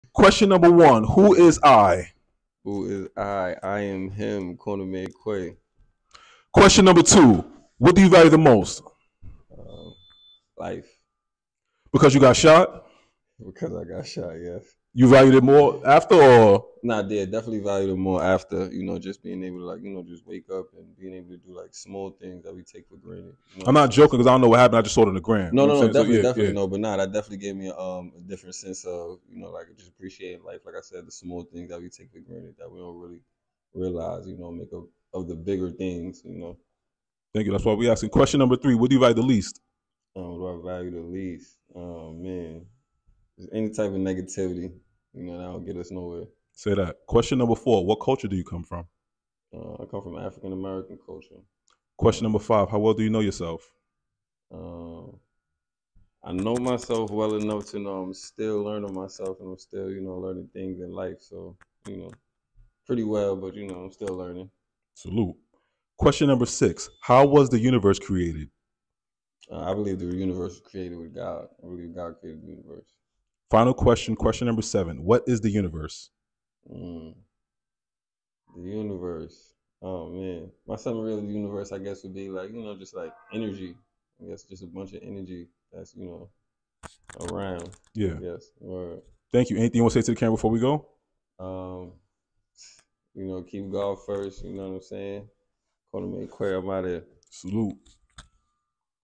Amar Bennett
At the Lit in the city event in Harlem NYC.